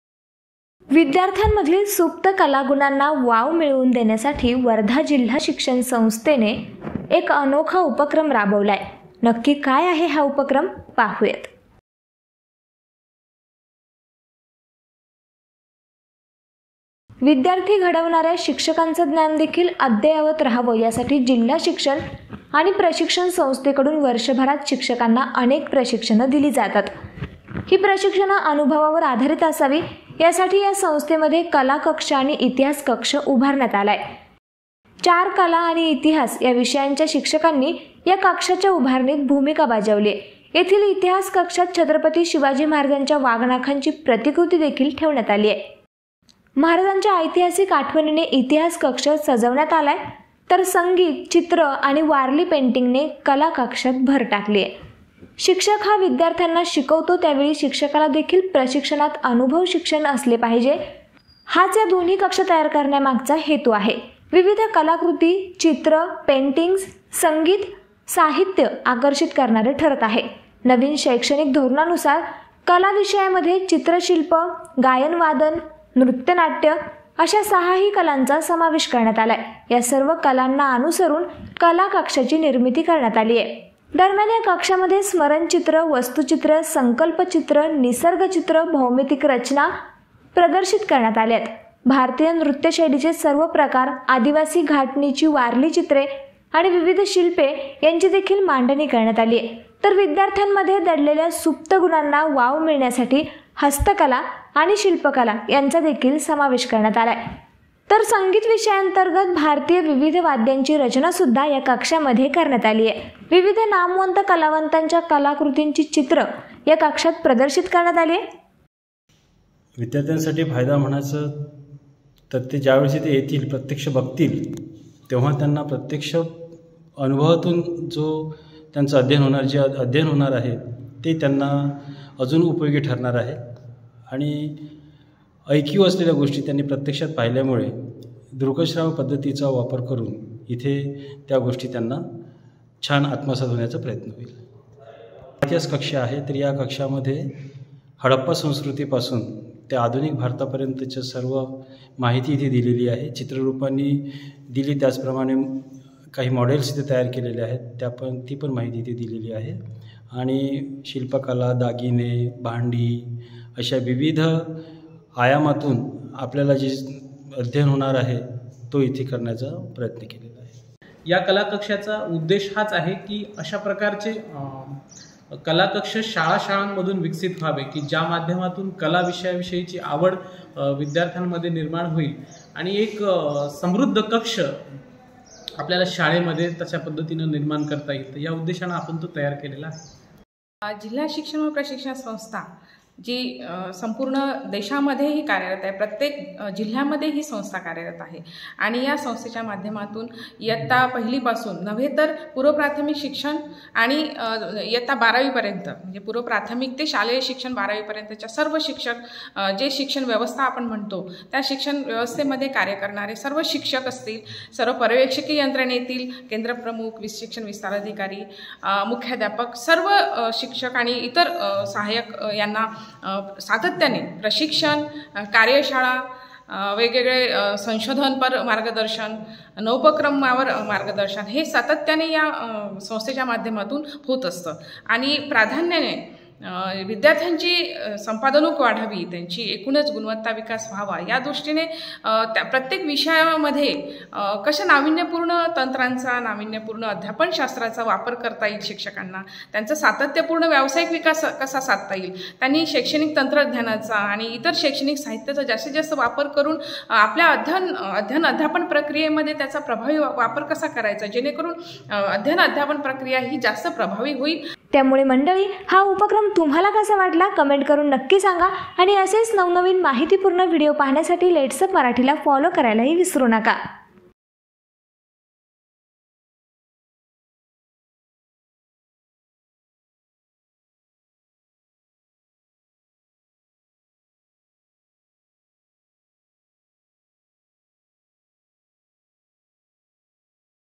Wardha News : वर्ध्यातील कला कक्ष पाहून तुम्ही व्हाल थक्क... | LetsUpp Marathi
वर्ध्यात शिक्षणासाठी कला कक्षांची उभारणी करण्यात आली आहे. विद्यार्थ्यांमधील सुप्त कलागुणांना वाव देण्यासाठी हा प्रयत्न आहे. असा उपक्रम राबविणारा वर्धा पहिला जिल्हा आहे.